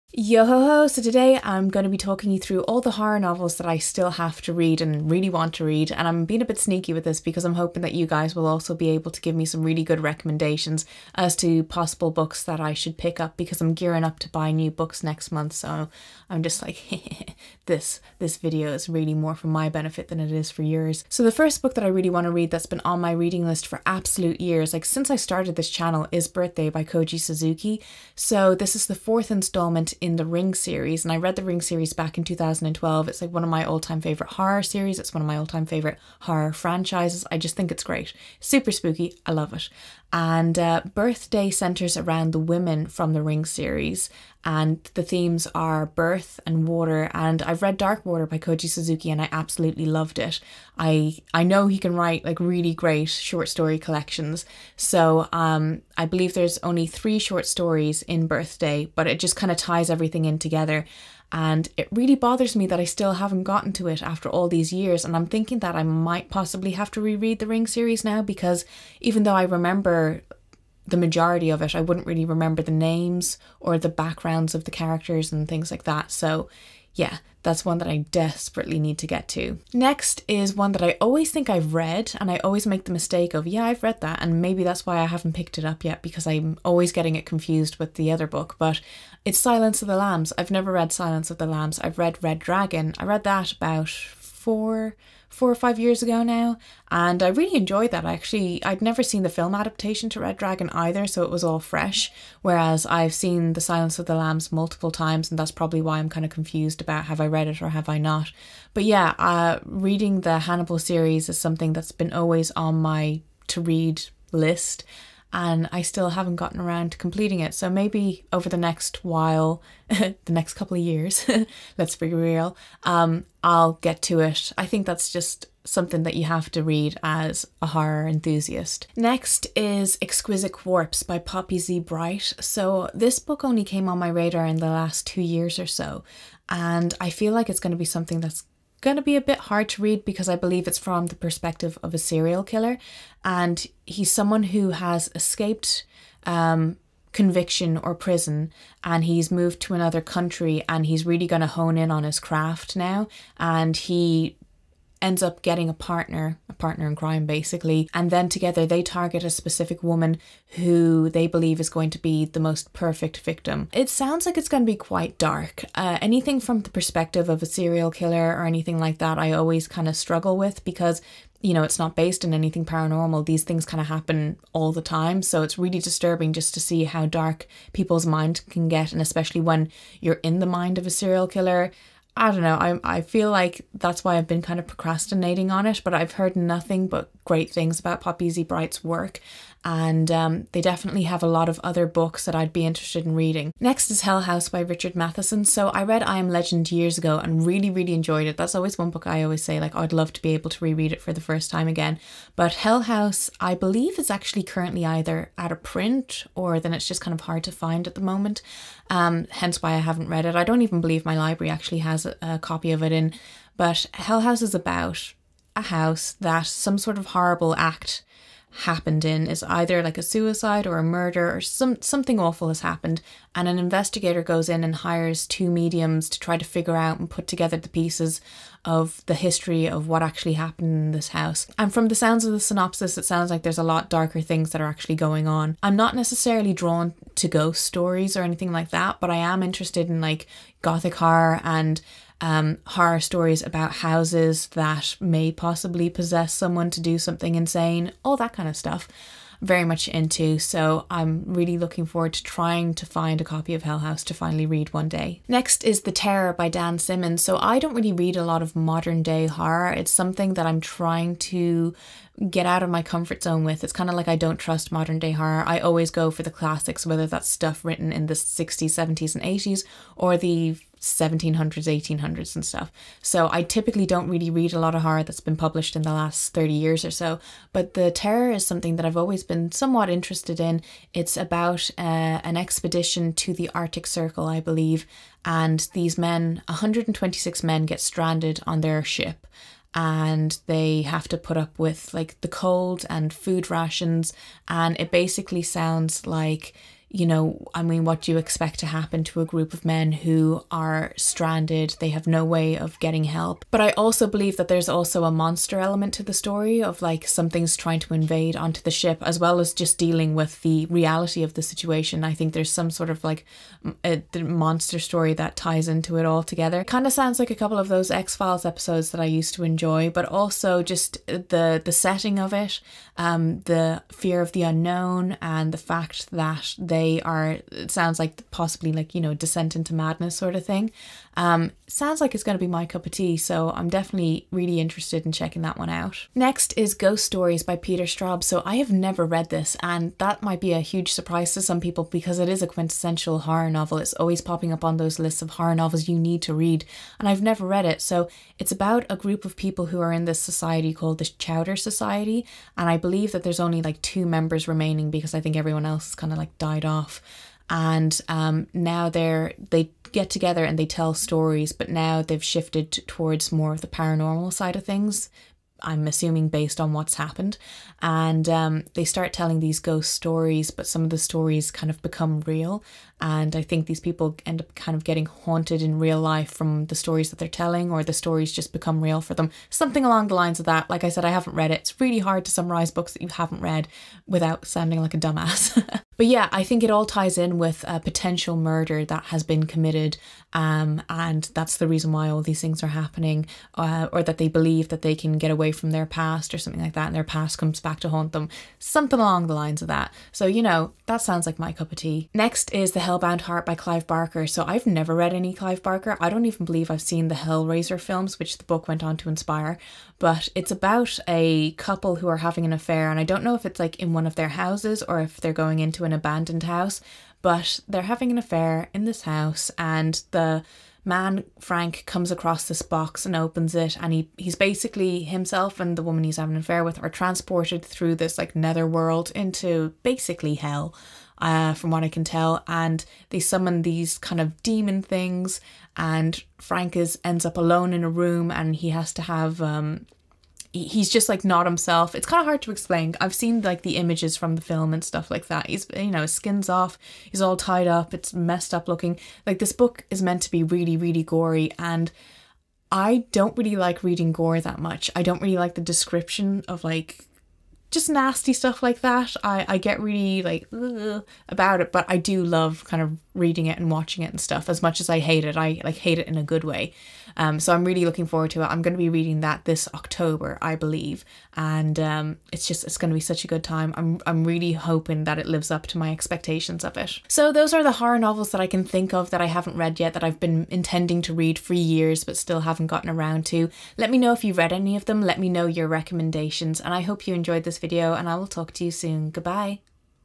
Horror Novels I Still Need to Read 💀
Books Mentioned
Birthday - Koji Suzuki
The Silence of the Lambs - Thomas Harris
Exquisite Corpse - Poppy Z. Brite
Hell House - Richard Matheson
The Terror - Dan Simmons
Ghost Story - Peter Straub
The Hellbound Heart - Clive Barker

🎬 FILMING EQUIPMENT 🎥
Camera - Panasonic Lumix DMC-FZ1000
Mic - Rode NT1-A
Interface - Yamaha Audiogram 3